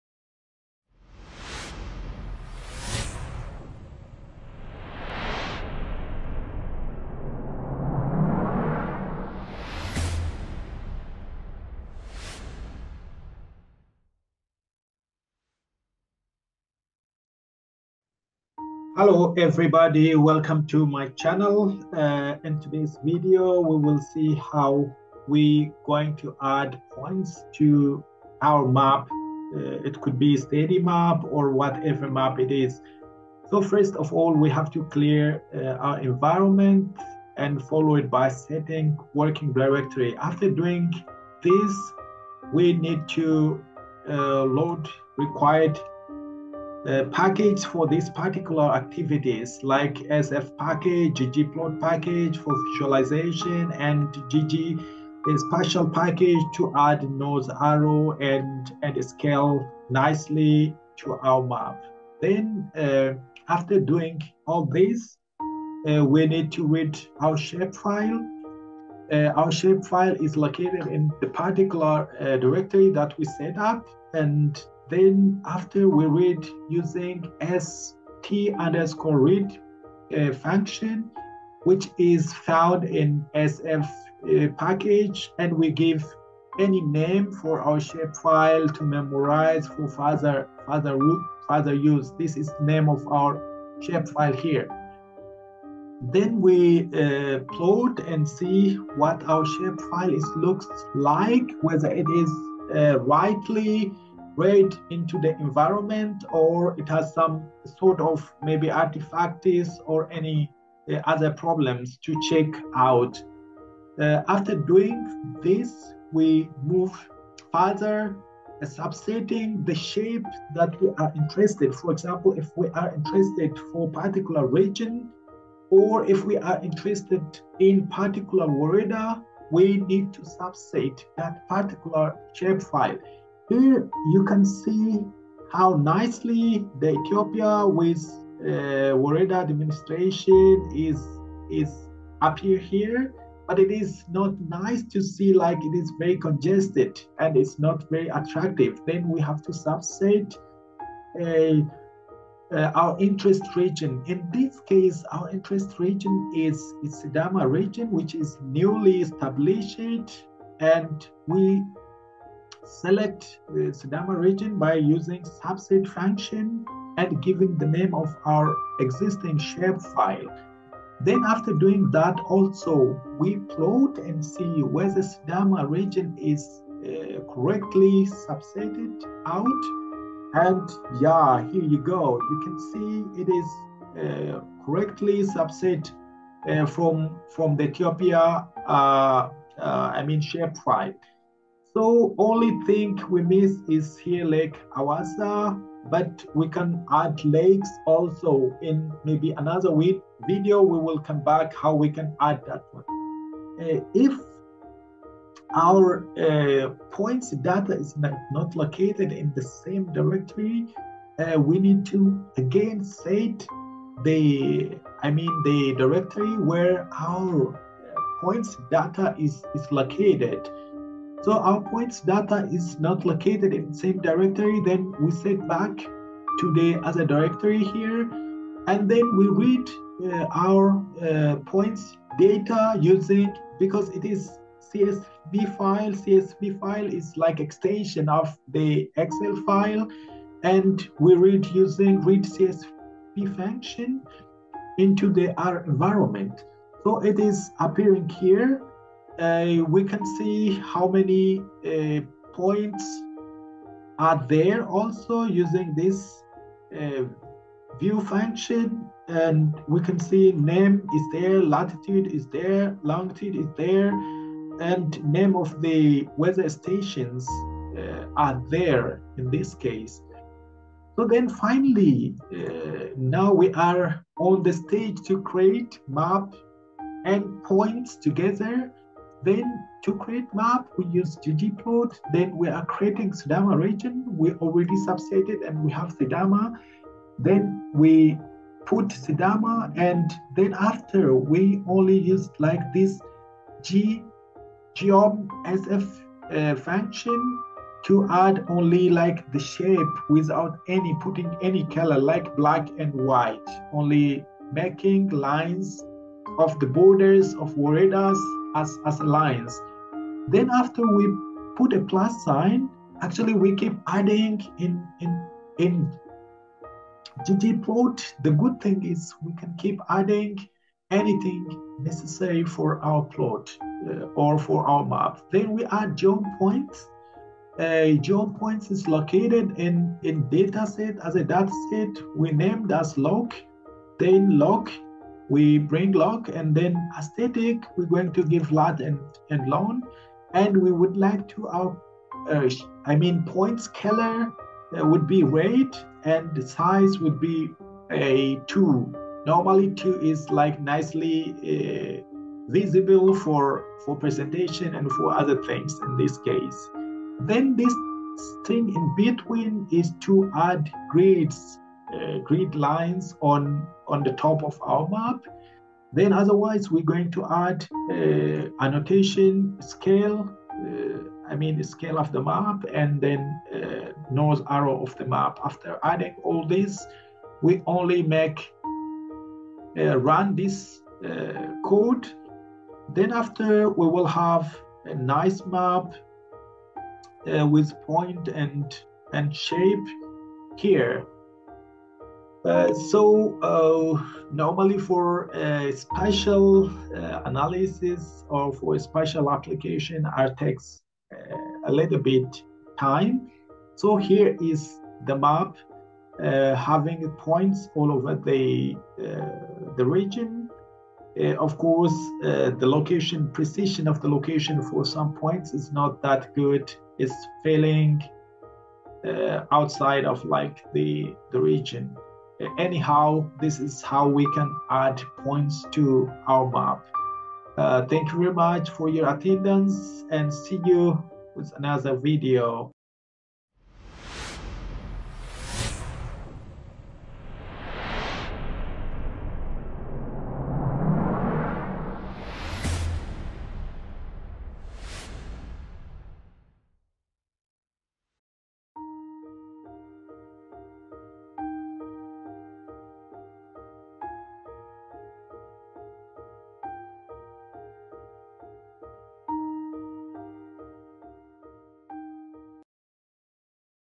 Adding points on the map
Adding points on the map using R programming.  I provide a tutorial on R basics in the other videos.  I also demonstrate how to use dplyr and ggplot to show data manipulation and data visualization. In the next videos, I will present how to plot basins, rivers, and other shape files.
You can add to the comments if you have any.